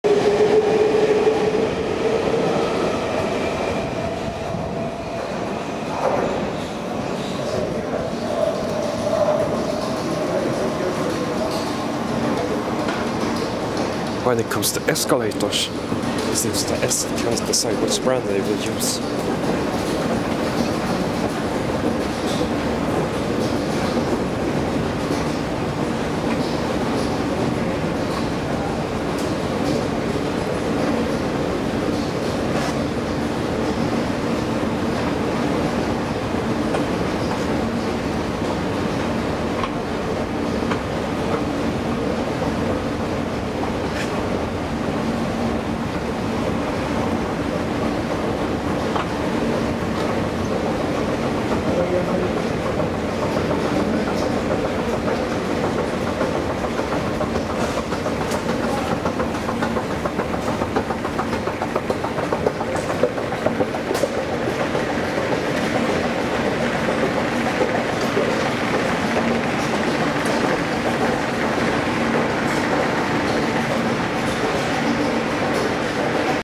2012-10-24 Escalator Replacement Update @ Bergshamra Metro Station
Update of the escalators being replaced in Bergshamra Metro Station recorded on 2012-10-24.
From: 1974 Hansa Fahrtroppen Escalators
To: 2012 KONE Escalators